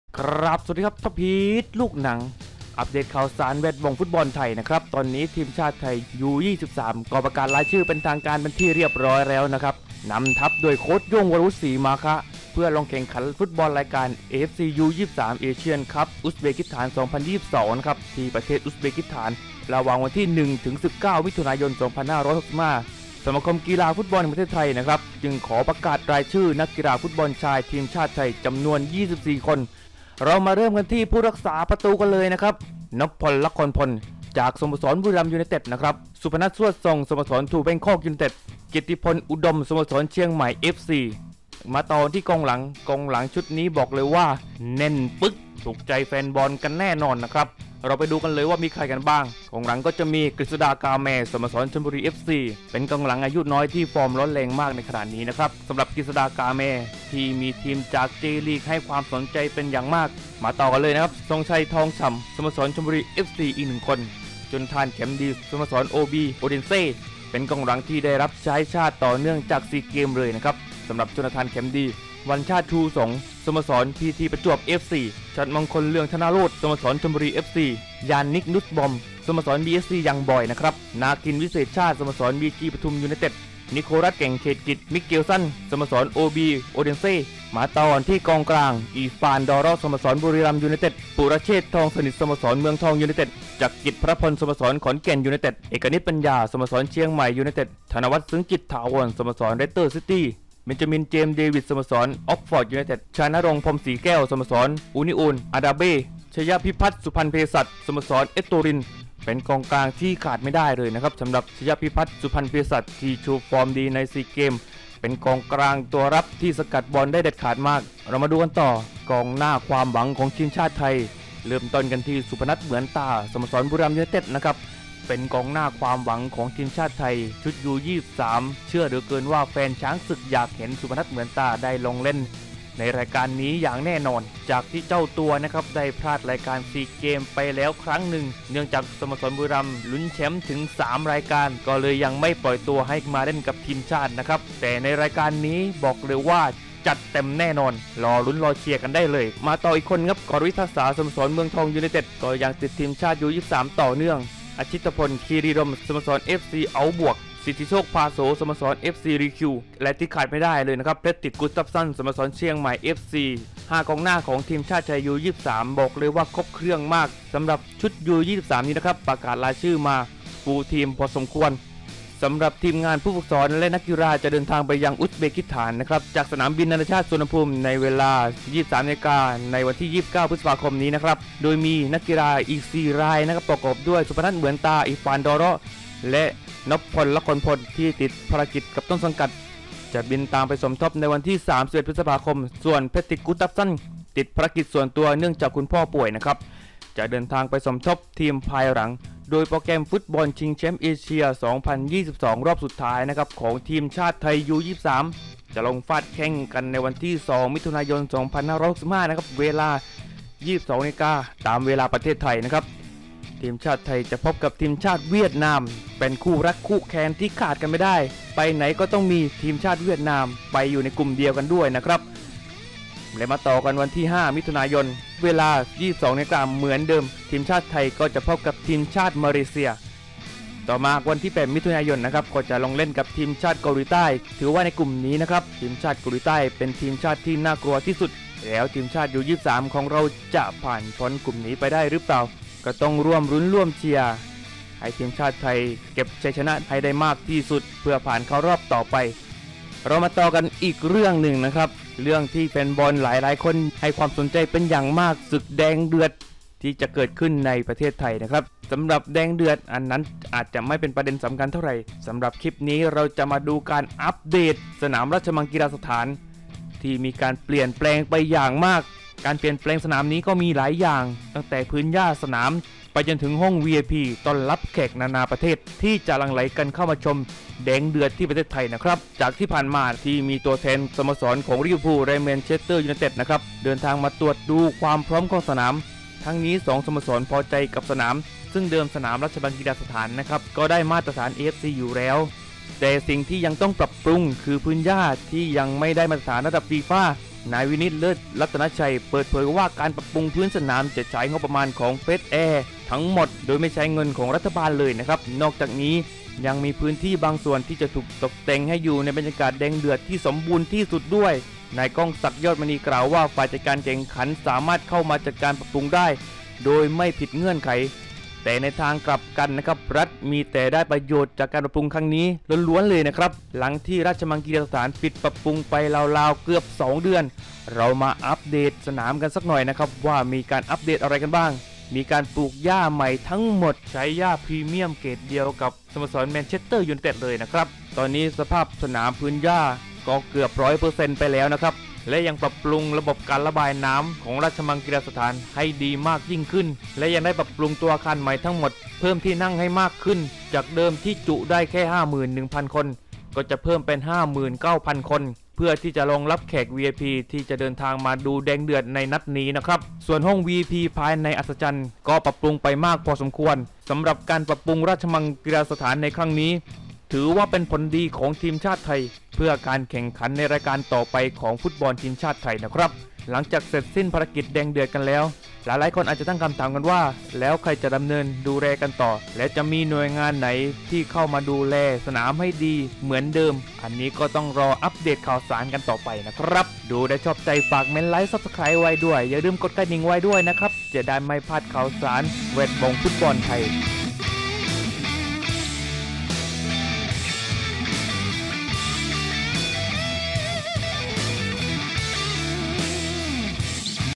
ช้างศึกยู23 ประกาศรายชื่อ 24 แข้งสนั่นเอเชีย โหดกว่าซีเกมส์ แน่นอน!!สนามราชมัง หญ้าสวยระดับโลก
1ซับ 1LIKE = คุณได้สนับสนุนผมแล้วครับ

เพจท็อปฮิตลูกหนัง